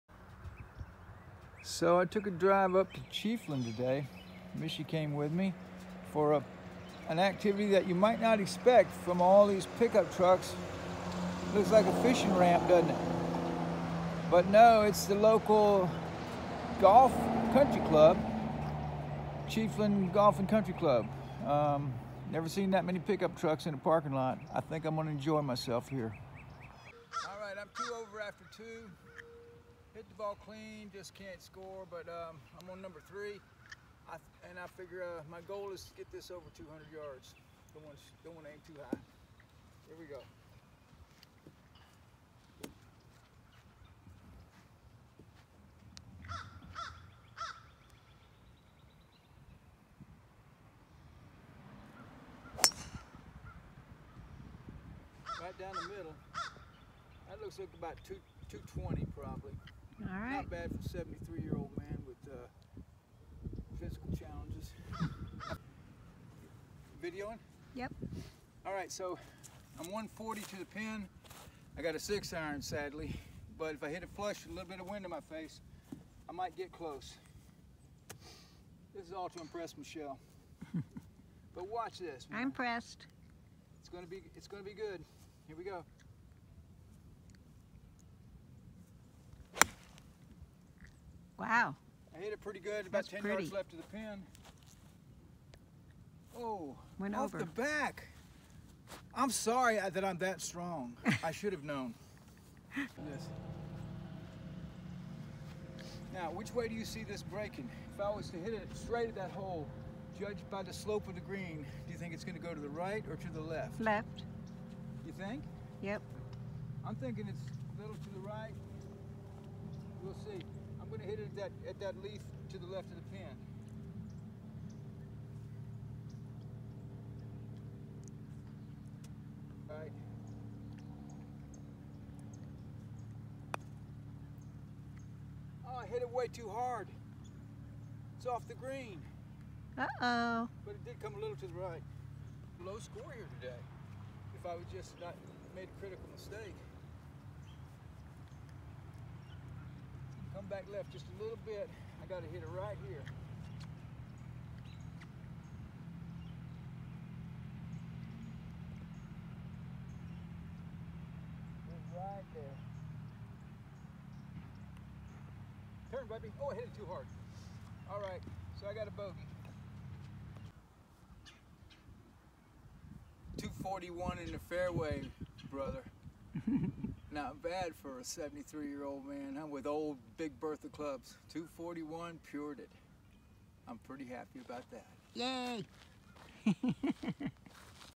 January 2023 Golf day in Chiefland!
Haven't played golf for a long time.  Mish and I read about this course online and decided to take a short road trip to check it out.  Had a great time!